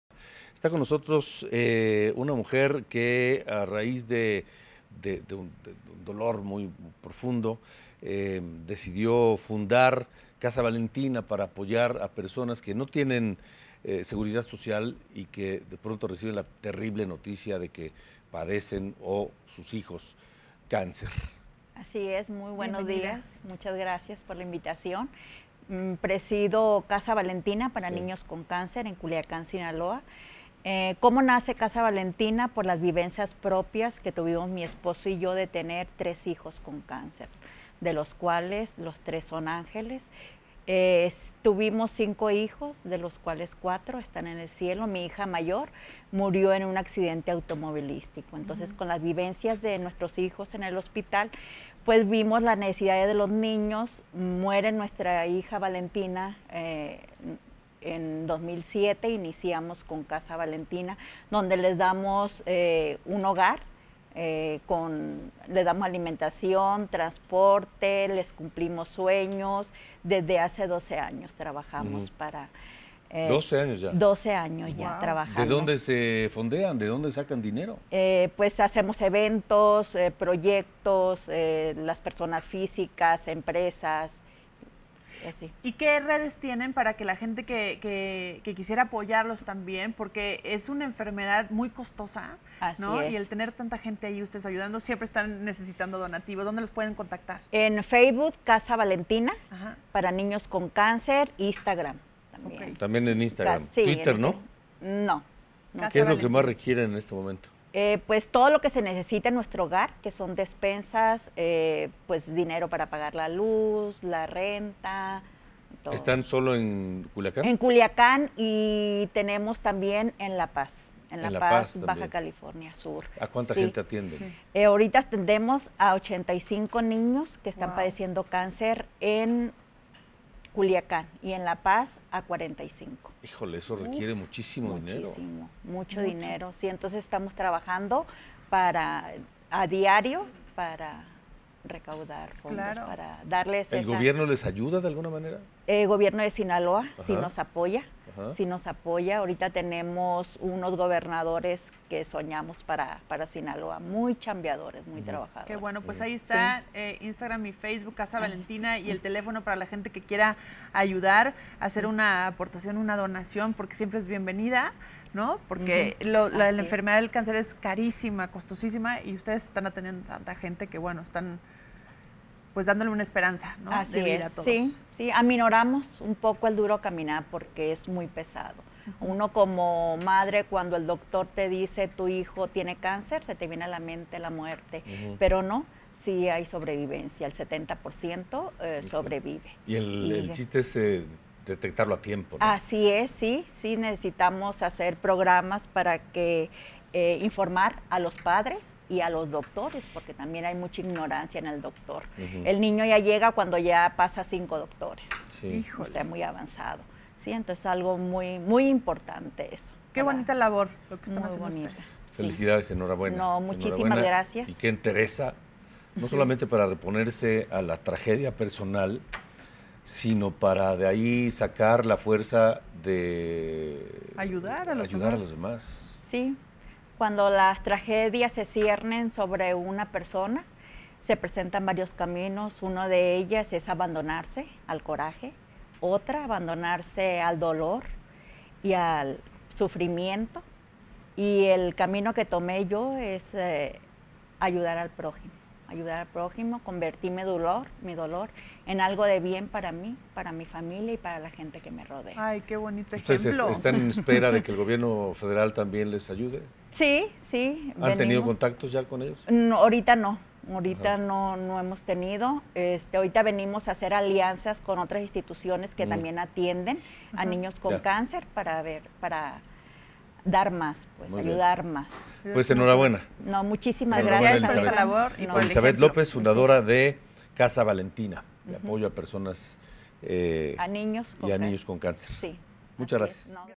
Elizabeth perdió tres hijos por el cáncer; hoy ayuda a otras víctimas
"Aminoramos el duro caminar", es como Elizabeth Rojo define la misión de Casa Valentina para niños con Cáncer, una asociación que lidera junto con su esposo en apoyo a los menores de edad con este mal.